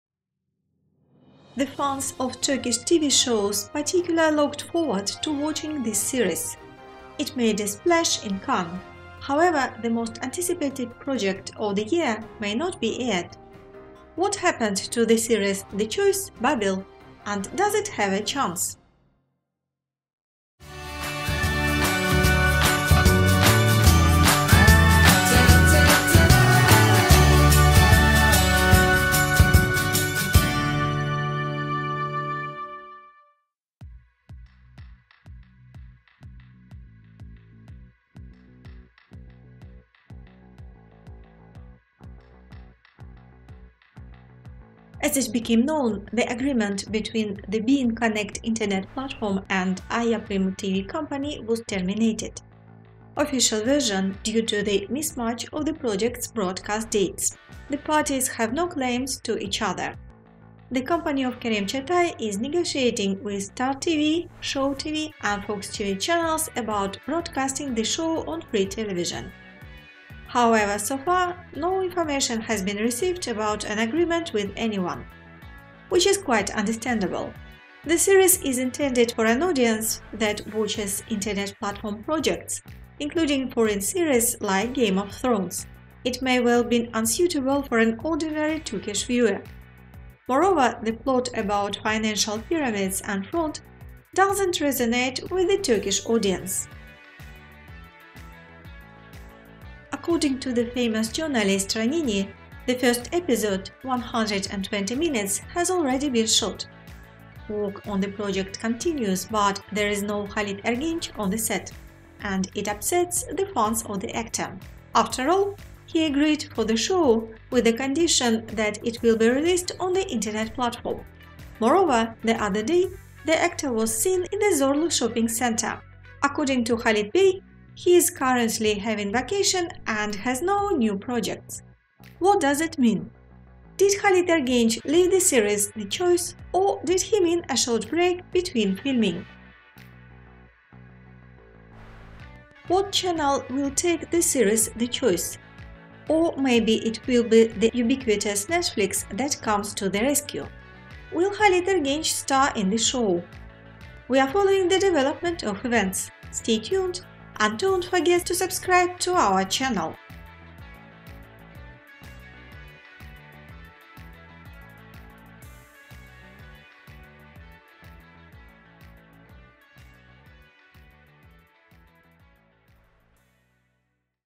Does Halit Ergenç’s upcoming serial get cancelled?
The fans of Turkish TV shows particular looked forward to watching this series. It made a splash in Cannes. However, the most anticipated project of the year may not be aired. What happened to the series The Choice / Babil and does it have a chance?